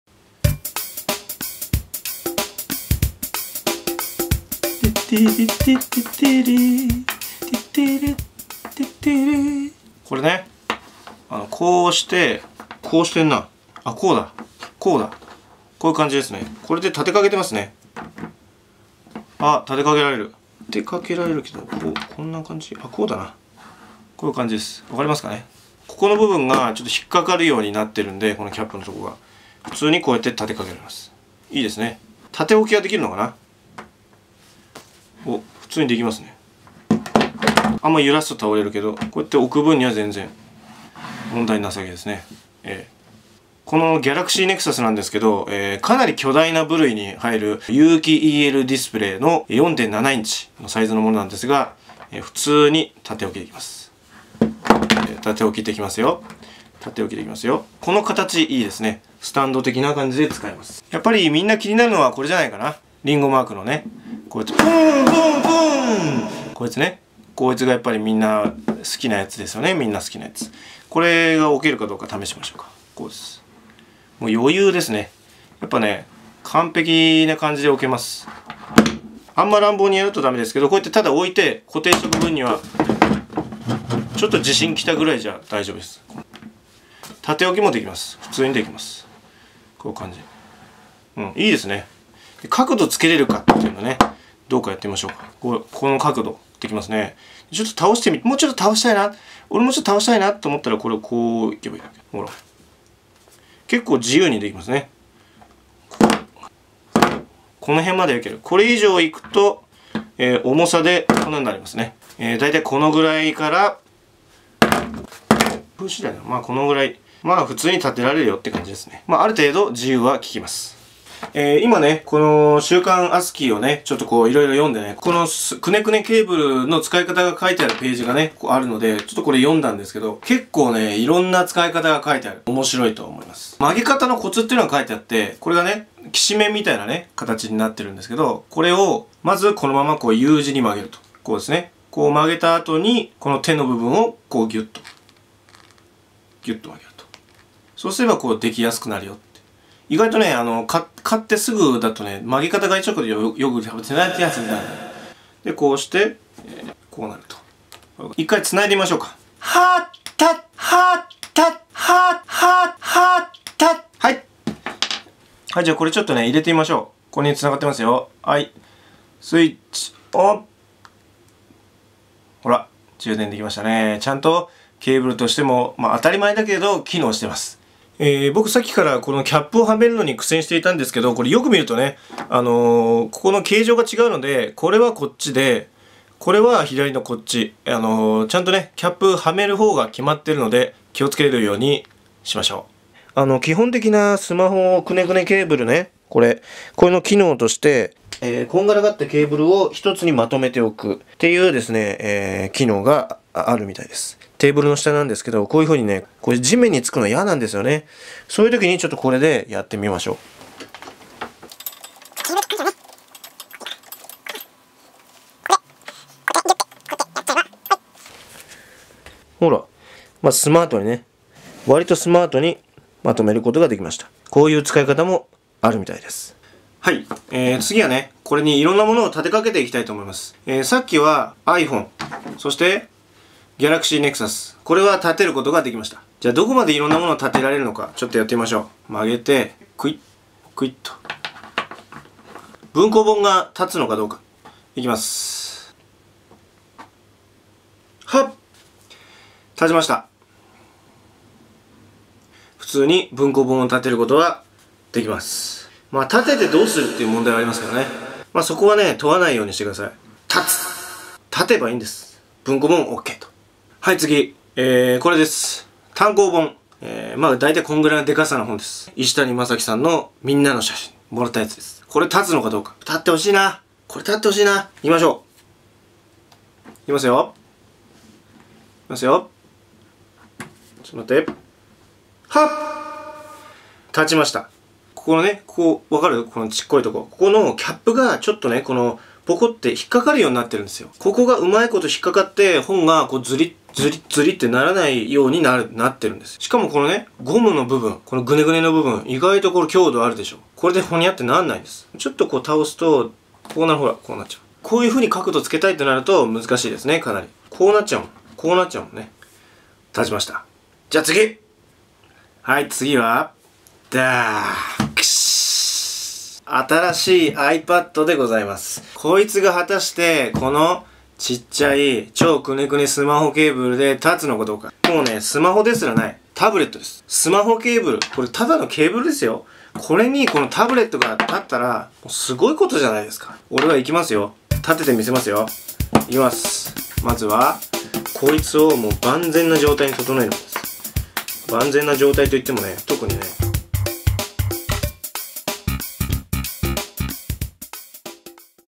超くねくねスマホケーブルがやってきた！その２
週刊アスキー５／１増刊号の付録「超くねくねスマホケーブル」のレビューです。